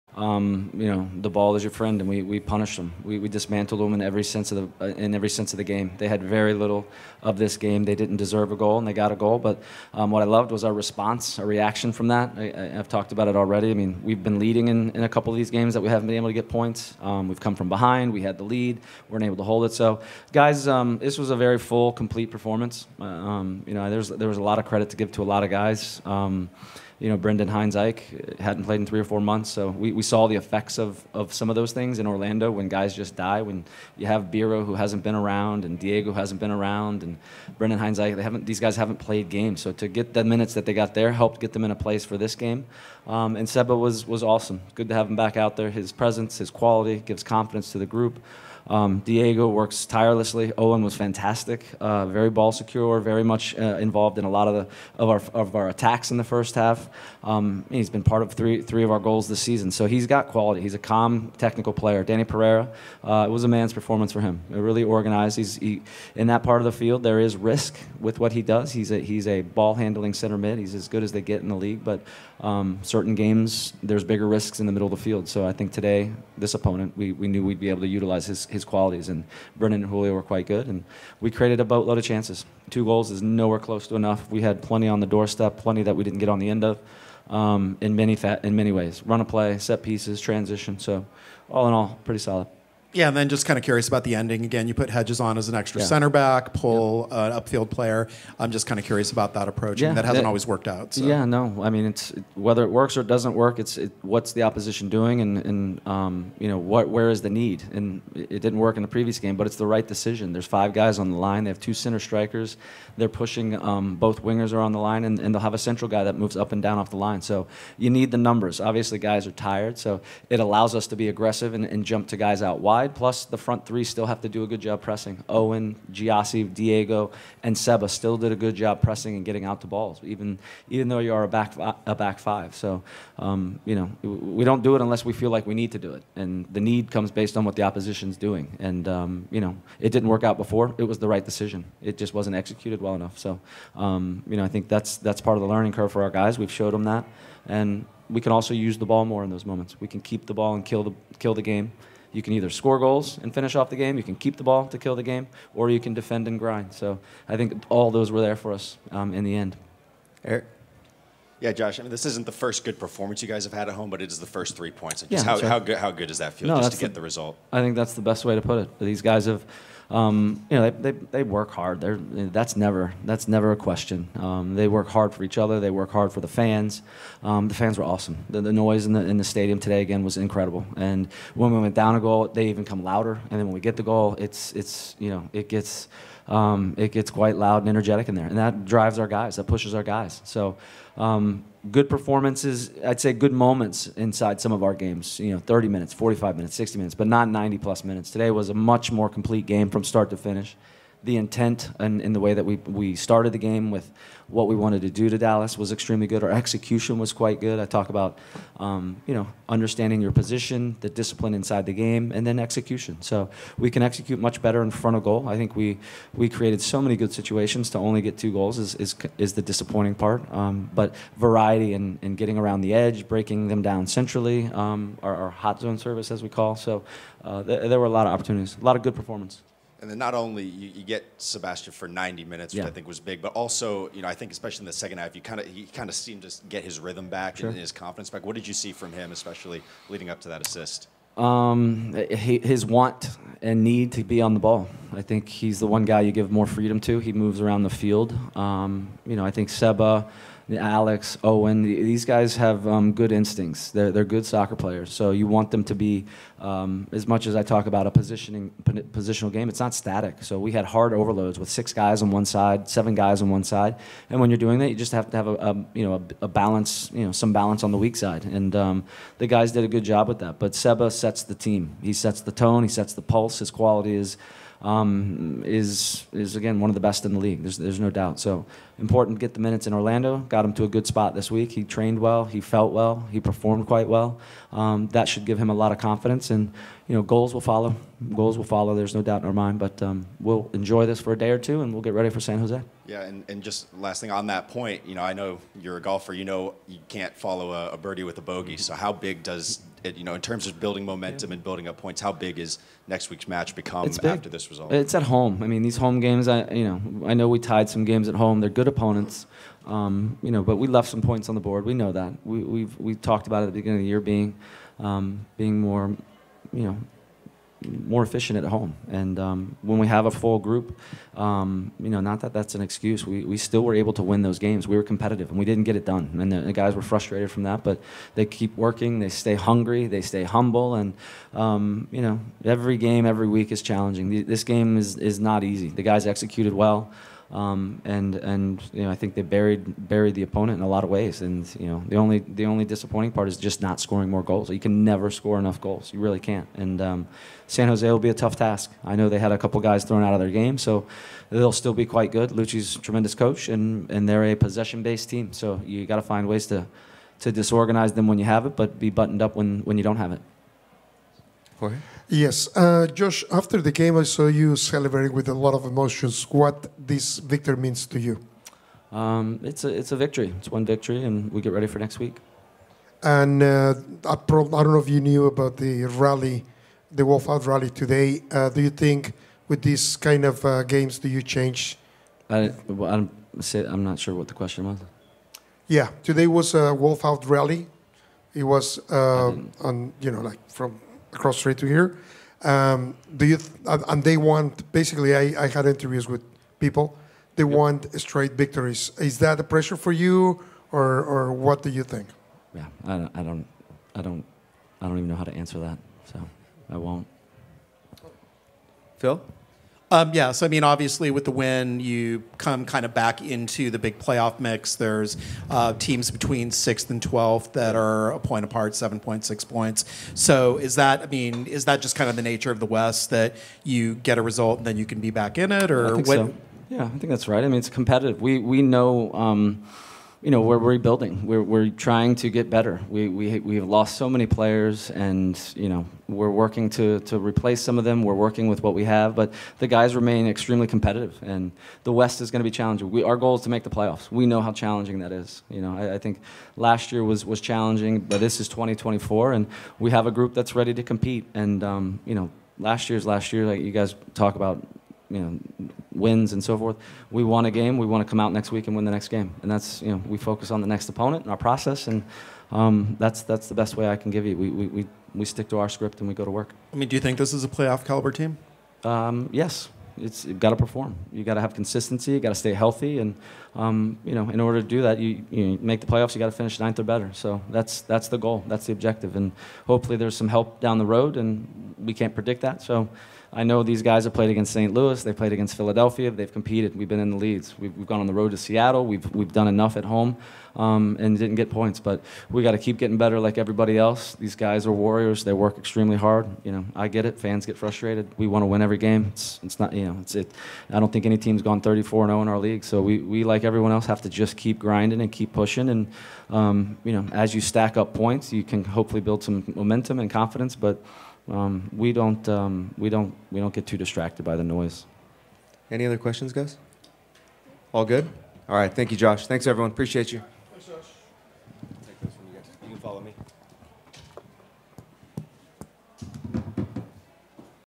Austin FC Post-Match Press Conference with Head Coach Josh Wolff - March 30, 2024
Austin FC Head Coach Josh Wolff speaks following the team's 2-1 home win over FC Dallas.